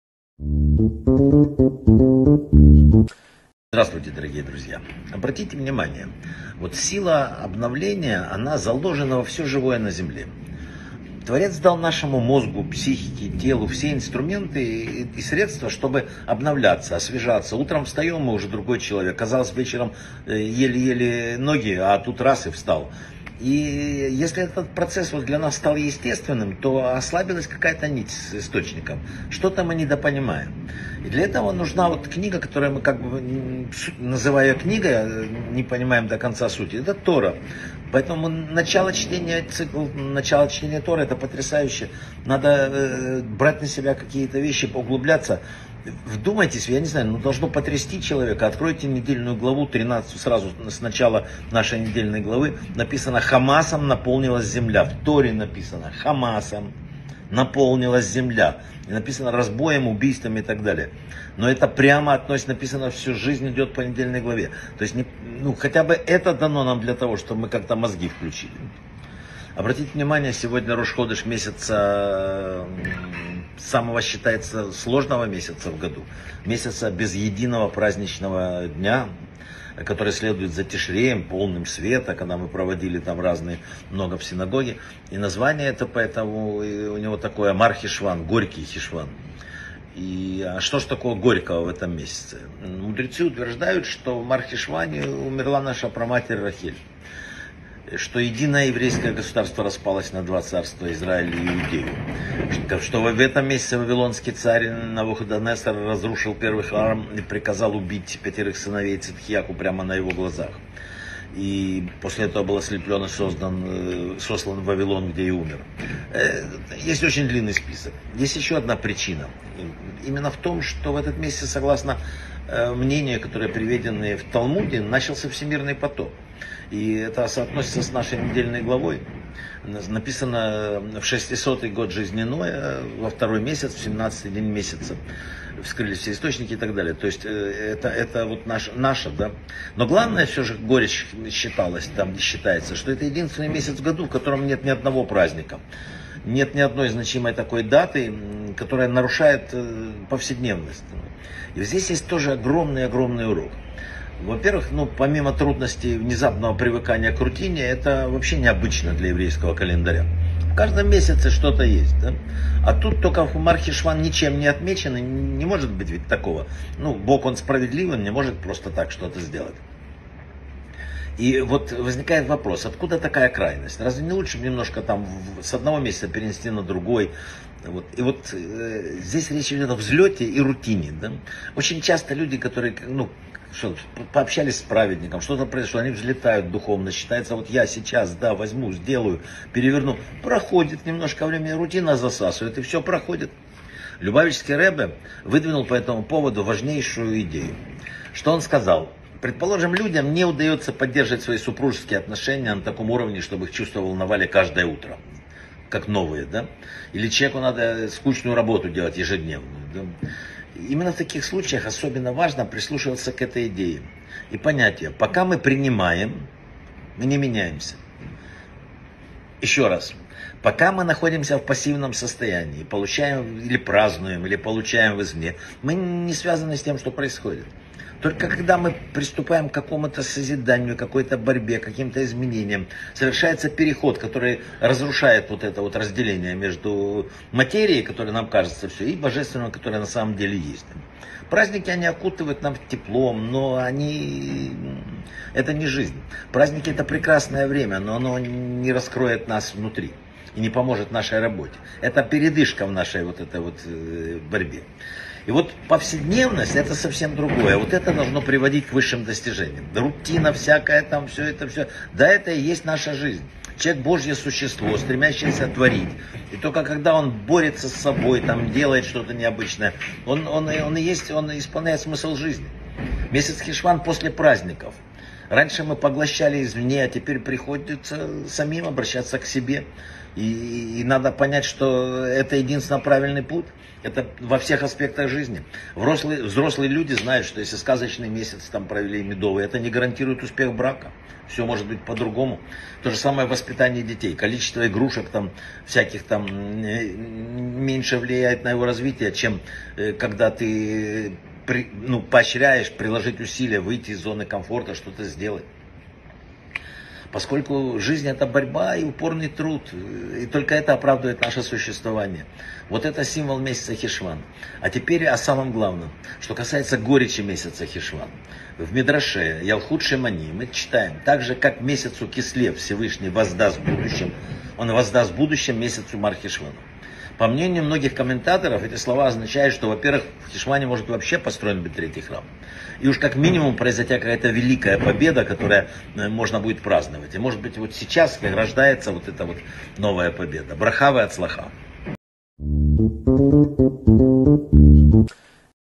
📘 Человек и рутина. Горечь месяца Хешван. Недельная глава Ноах - Урок 2 | Вадим Рабинович
Человеку, как и всему живому, дана удивительная возможность обновления. Почему опасно превращать этот процесс в нечто рутинное и само собой разумеющееся?
Тора - больше чем просто книга, это инструмент восстановления нашей связи с источником.
Какой намек на сегодняшние события встречает нас в нашей недельной главе?
Месяц Хешван - его суть и символика. Что говорят о произошедшем в этом месяце мудрецы? Когда, согласно Талмуду, произошел Всемирный потоп?
Роль праздников как дней, выделяющихся из повседневности. Почему в нашем календаре есть такие крайности? Идея духовного взлета и спуска в рутину.
Что говорил на эту тему Любавичский Ребе? Отдача и борьба - ключ к изменениям.
Истинная жизнь человека - в ежедневных усилиях, ведущих к достижениям.
Примеры из семейной жизни и воспитания детей, проливающие свет на то, что после приятных моментов обязательно приходит время работы.